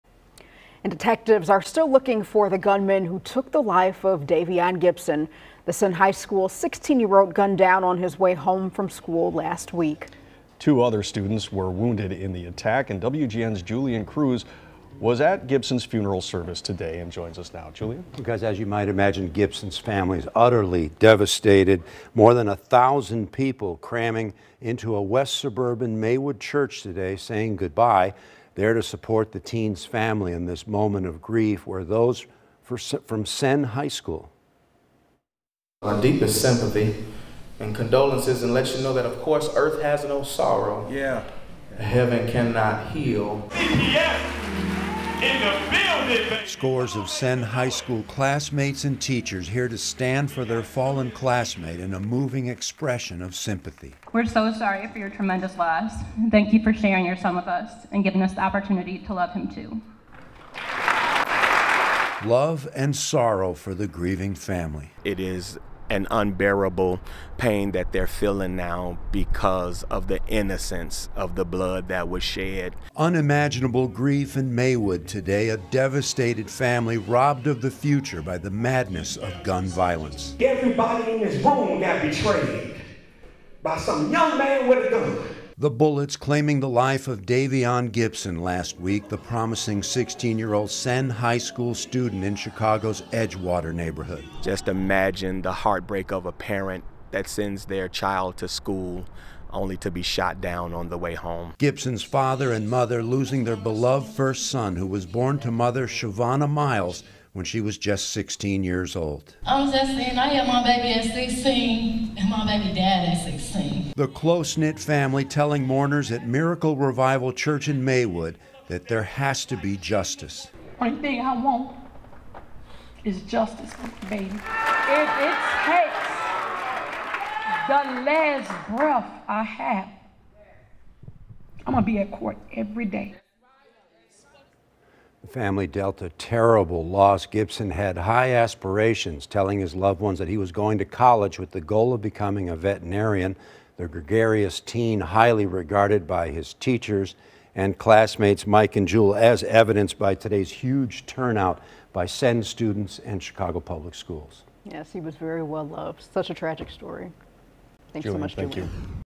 Funeral services held for 16-year-old Senn High student gunned down near school
Hundreds of people gathered inside a west suburban Maywood church on Friday to say goodbye to the 16-year-old Senn High School student gunned down last week after school.